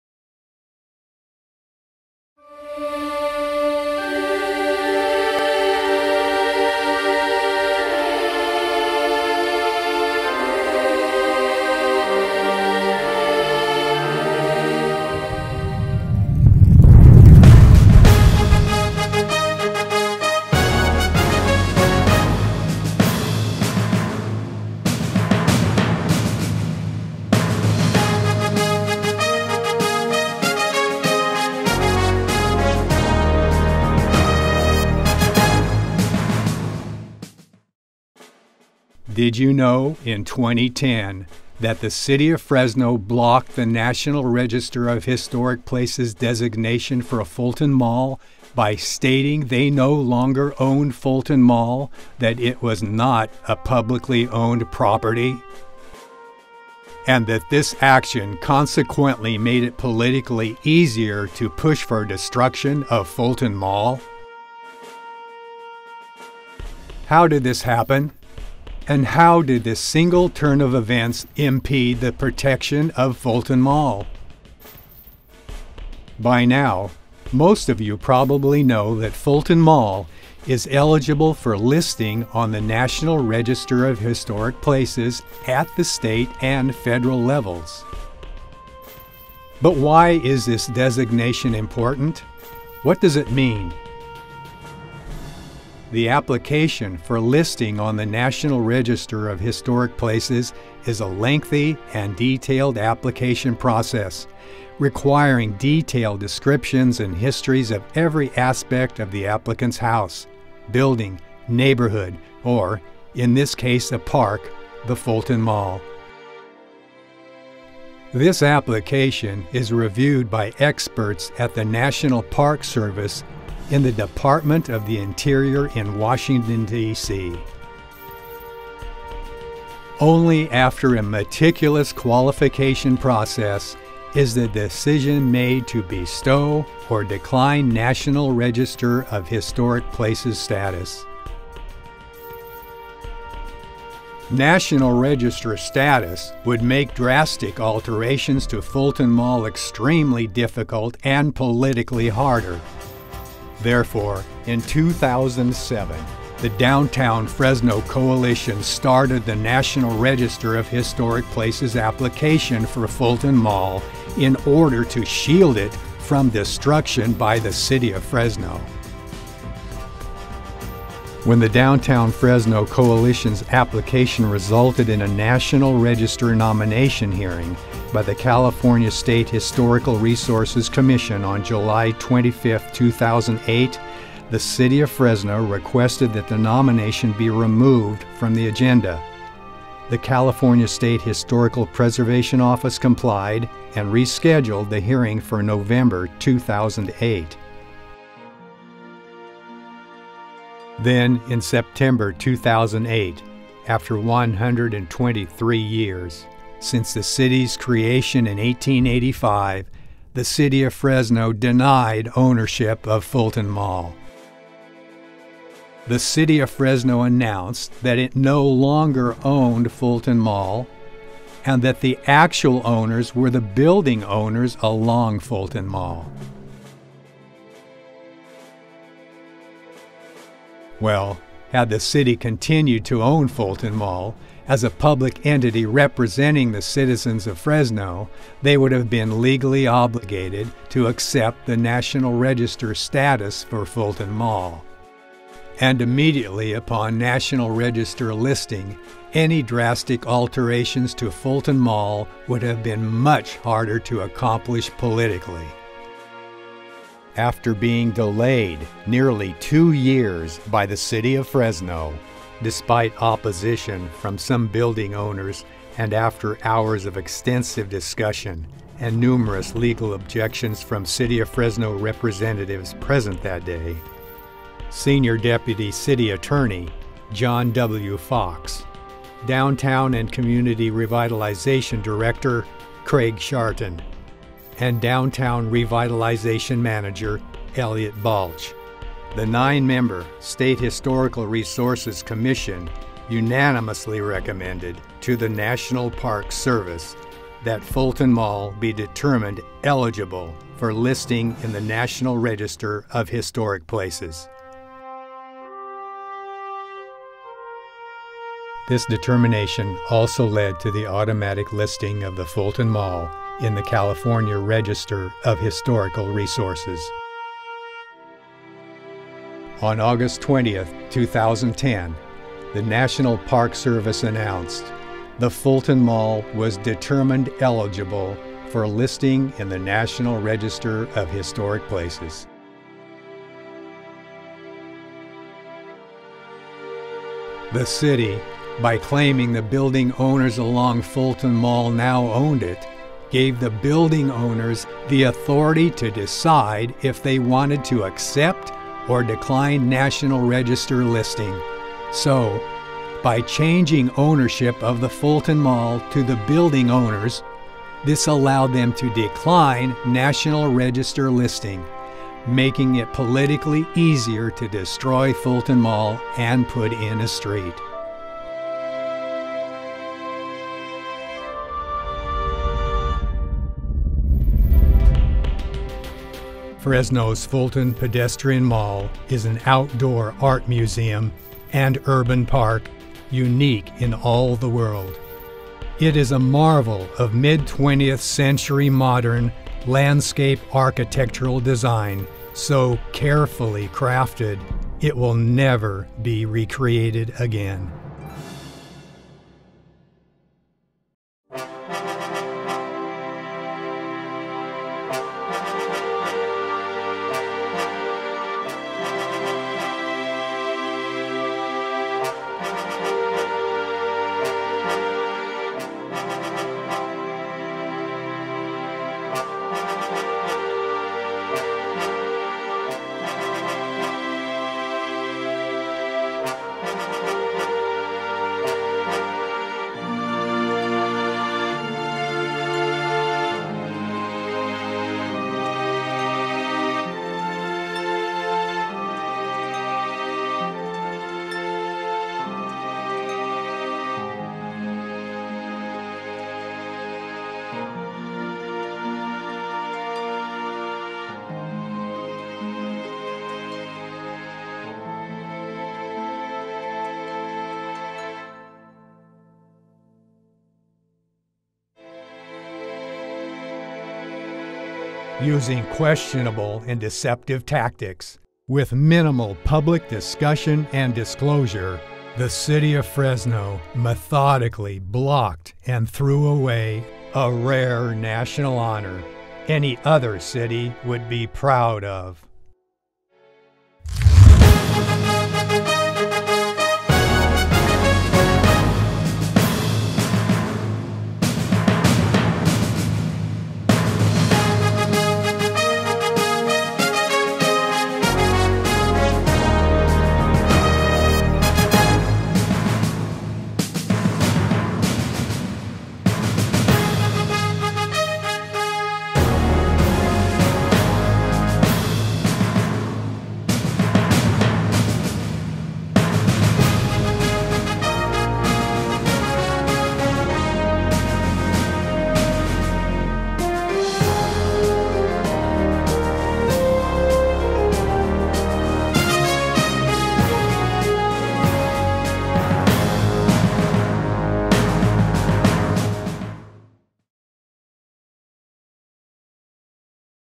Politicians Killed the Fulton Mall Natl Historic Places Listing - Part of "Look Around, Follow Me"
The 16-part Fresno Fulton Art Mall Documentary
“Look Around, Follow Me” continues chronicling the world’s largest and most valuable “Mid 20th Century Modern” outdoor public pedestrian art park.

Did you know Fulton Art Mall is eligible for listing on the National Register of Historic Places?  The only reason it is not listed is because the City of Fresno, led by Mayor Ashley Swearengin, methodically blocked and threw away the National Register of Historic Places listing for Fresno's Fulton Art Mall.

The reason?
To diminish the importance of the Fulton Art Mall and eliminate any protection the National Register of Historic Places Listing could provide to deter the destruction of Fresno's Fulton Art Mall.

How did the City of Fresno do this?  Watch the film and Discover the despicable truth!